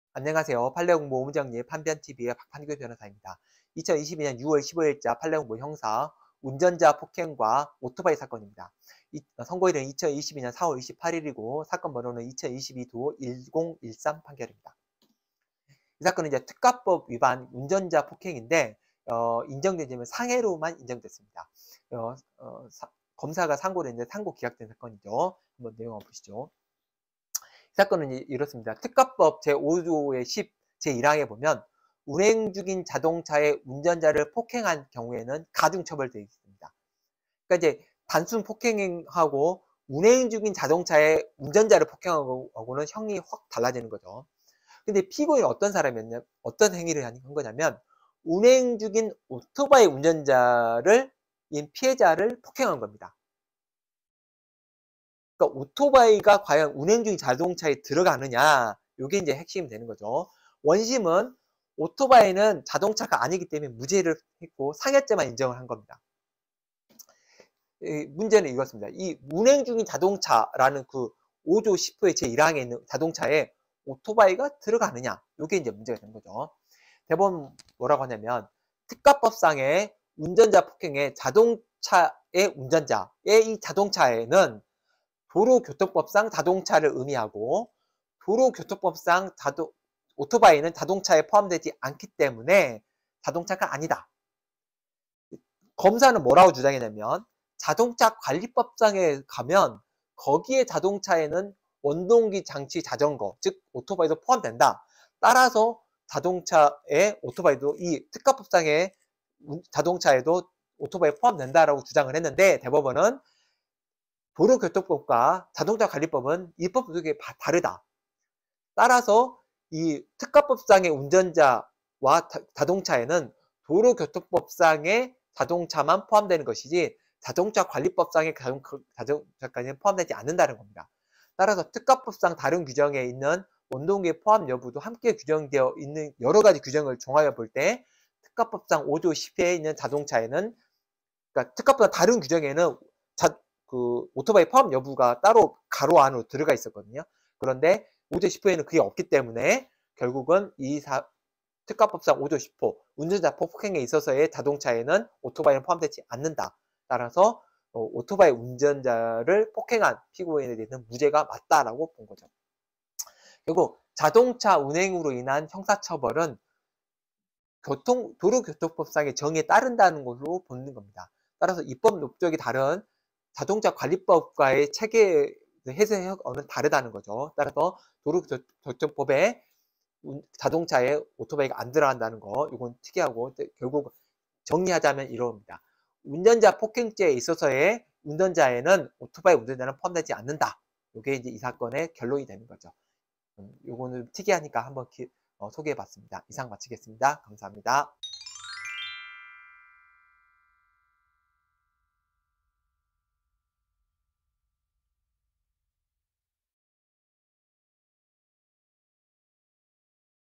운전자폭행과 오토바이 사건 [22.6.15.자 판례공보(형사)]
운전자폭행과 오토바이 사건
별 4개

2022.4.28. 선고 2022도1013 판결
특가법위반(운전자폭행등)(인정된 죄명 상해)

상고인 검사

상고기각

특가법 제5조의10 제1항 “운행 중인 자동차의 운전자를 폭행~~”
피고인이 운행 중인 오토바이 운전자인 피해자를
폭행하여 상해를 가한 것으로 기소됨
원심은 오토바이는 자동차가 아니라며 무죄, 상해죄 인정

운행 중인 자동차에 오토바이가 포함되나?

특가법의 운전자폭행의 “자동차의 운전자”는
도로교통법상 자동차를 의미하고
도로교통법상 원동기장치자전거는 자동차가 아님
자동차관리법상 자동차에 이륜자동차가 포함되어 있으나, 입법 목적이 다름
특가법의 다른 규정에는 원동기 포함여부가 함께 규정되어 있음

실무활용
자동차 운행으로 인한 형사처벌은
도로교통법상의 정의에 따른다는 것
입법목적과 다른 규정과의 체계적 해석에 의한 것
운전자폭행의 운전자에 오토바이 운전자는 포함되지 않음